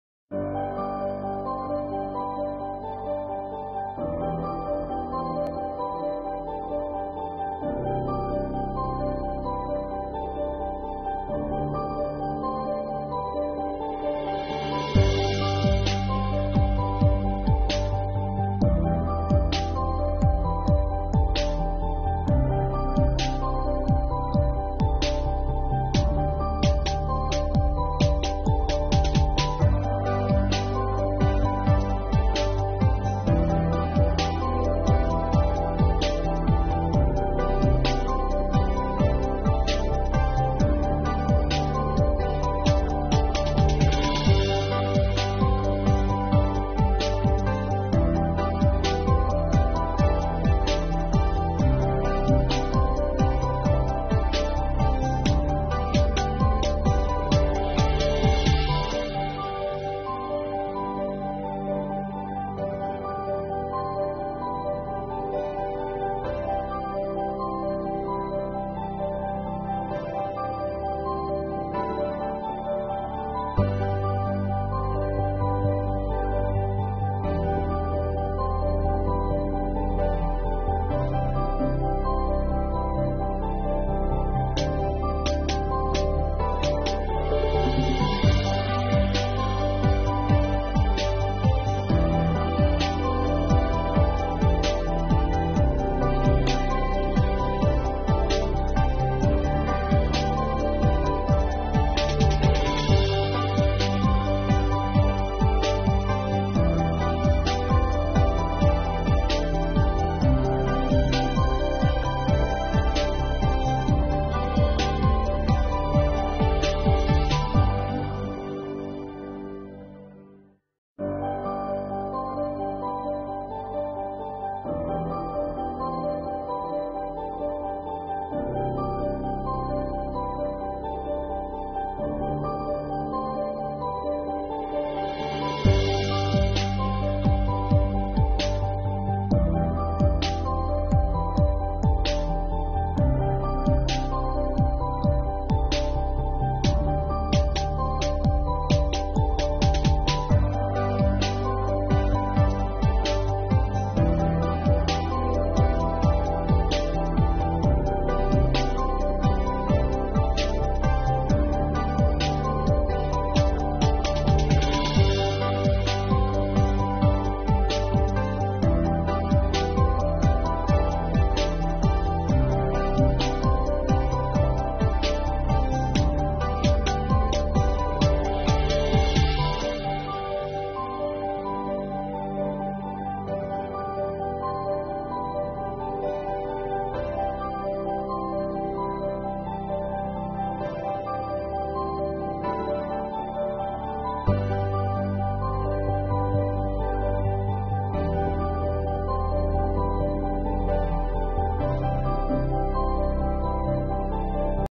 Kronce-Smargon solution: PET plant piping layout
Kronce PET preform injection molding system with robot, PET stretch-blow molding machine, run in the client's factory.